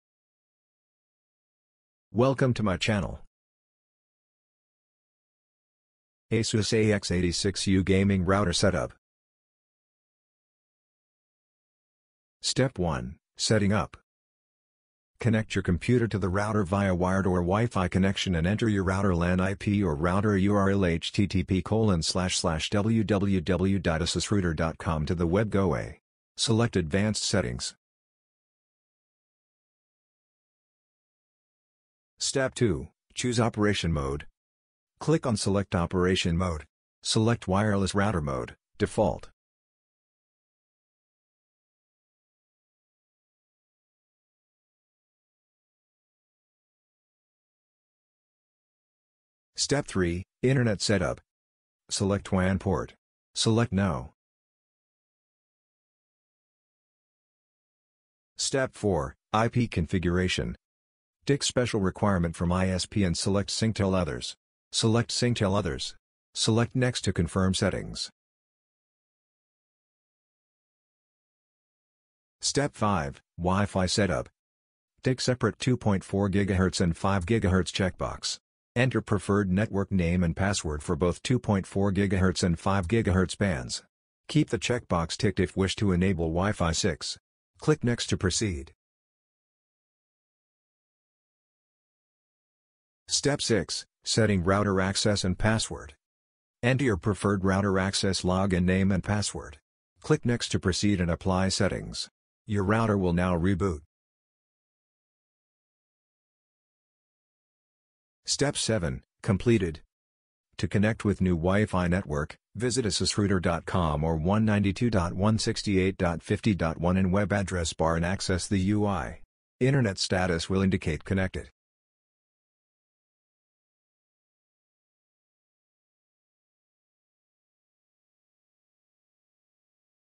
Asus AX86U Gaming Router Setup | asusrouter.com
Here all the details for Asus AX86U Gaming Router Setup available in the video. Check the chapters-

0:10 "Steps start for Asus AX86U Gaming Router Setup"
0:35 "Choose operation mode on the Login Page"
1:01 "IP configuration for the Asus AX86U router"
1:37 " Router access password settings"
2:07 "Technical Solution"

Complete solution is provided here for Asus AX86U Gaming router device. You can use this router for fast gaming experience. For more information related to router or extenders, you can get connected with us.